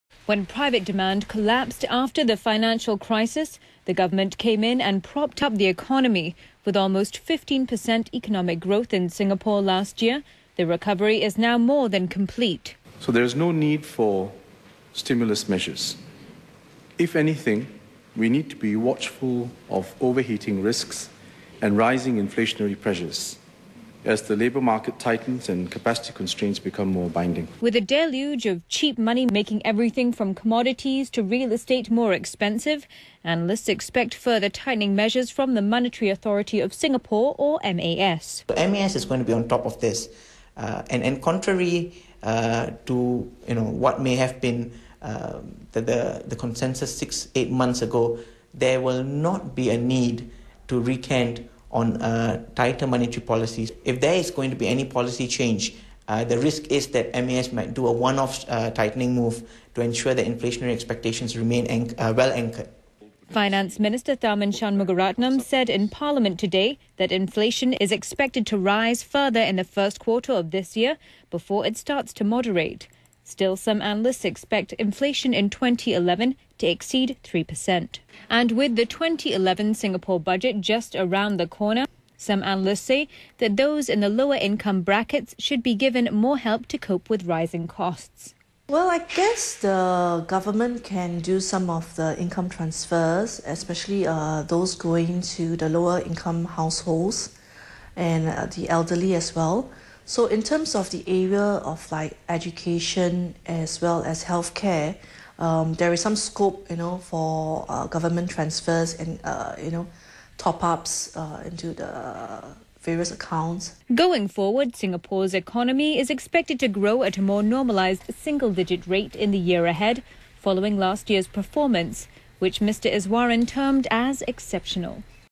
Singapore economy risks over heating
There would be no need for stimulus measures in Singapore this year, Senior Minister of State for Trade and Industry S Iswaran said in Parliament on Monday. First published Jan 10 2010.